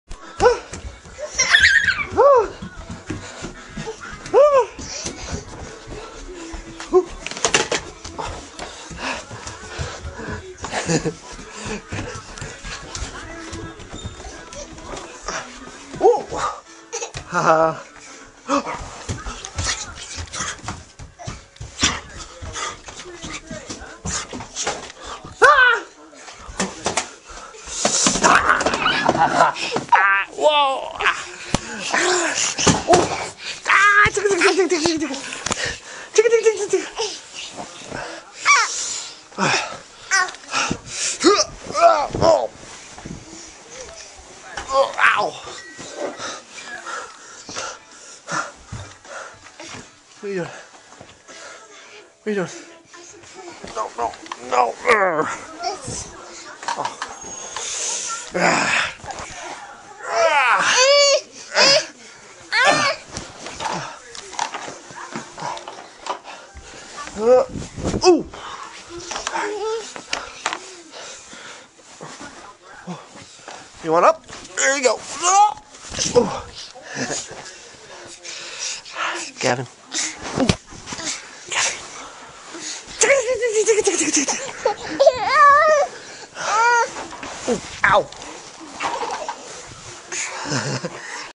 497 - THE CHASE (... and then Really Nothing)
Gavin chases me around the couch, and then really nothing. lol